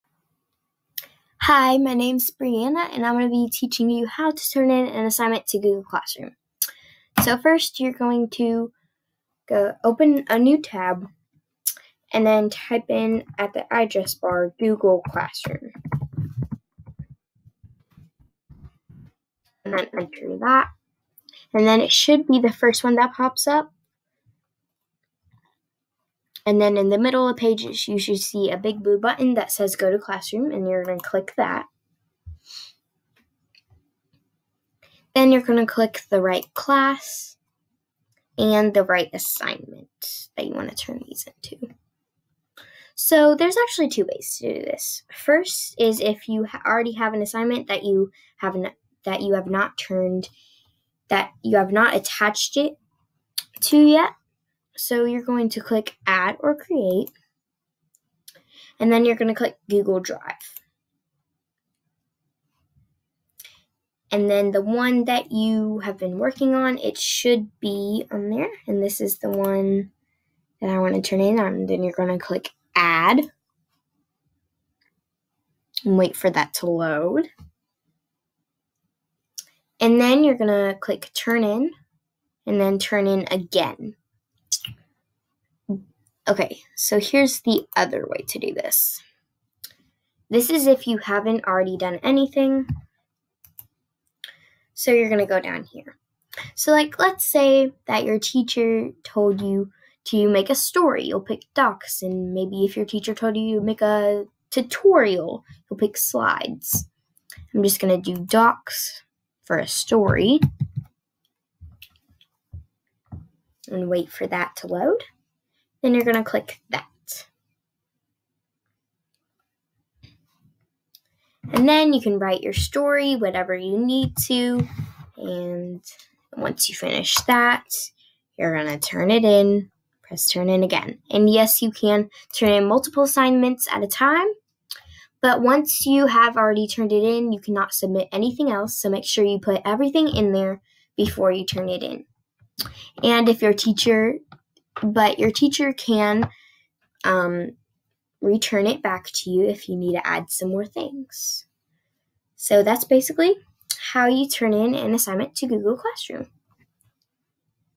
STLP Tutorial 2020: Uploading and attaching assignments
How to upload, and attached assignments in Google Classroom.